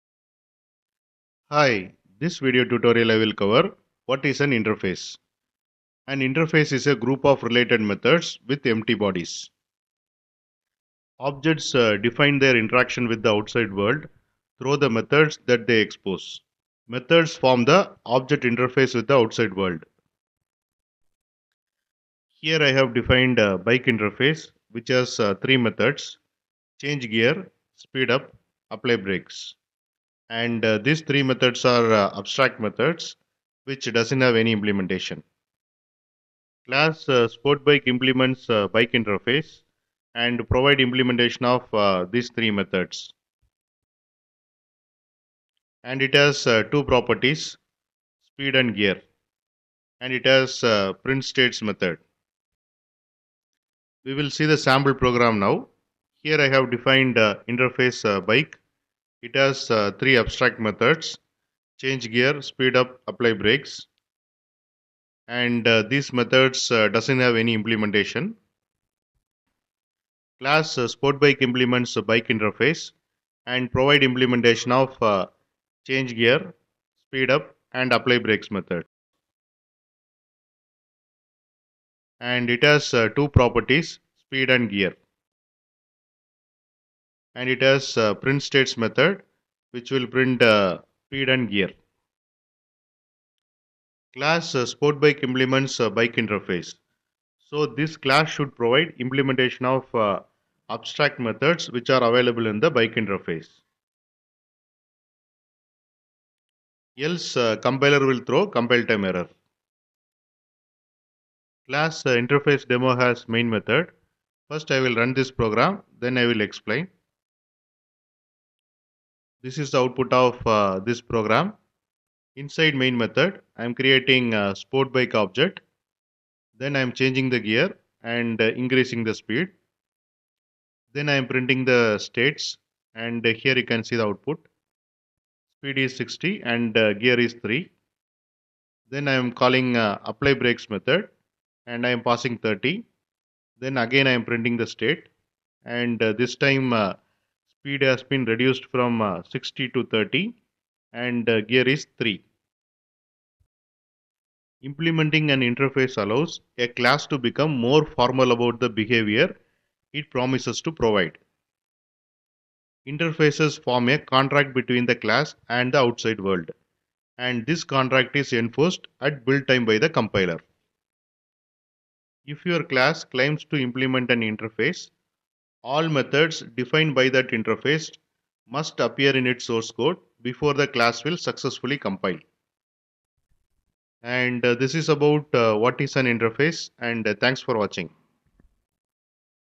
Java Interface Explained with Bike Analogy | Java Tutorial
In this enlightening Java tutorial, we dive into the concept of interfaces using a bike analogy, providing valuable insights and practical examples to deepen your understanding of this foundational programming concept.

Description:
Are you struggling to grasp the concept of interfaces in Java? Join us as we simplify the intricacies of interfaces using a relatable bike analogy, making it easier to understand and apply in your Java programming journey.

Interfaces play a pivotal role in Java development, enabling you to define a contract for classes that implement them. However, comprehending interfaces can be challenging for beginners. In this tutorial, we simplify the concept by likening it to a bike, making it more accessible and intuitive.

We delve into the fundamentals of interfaces, explaining how they serve as blueprints for classes and facilitate polymorphism in Java. By using the bike analogy, we illustrate how interfaces define a set of behaviors that classes can implement, much like the components of a bike contributing to its functionality.

Through clear explanations and hands-on examples, we guide you through the process of creating and implementing interfaces in Java, demonstrating their versatility and practicality in real-world scenarios.

Whether you're new to Java programming or seeking to reinforce your understanding of interfaces, this tutorial is designed to empower you with the knowledge and skills to leverage interfaces effectively in your projects.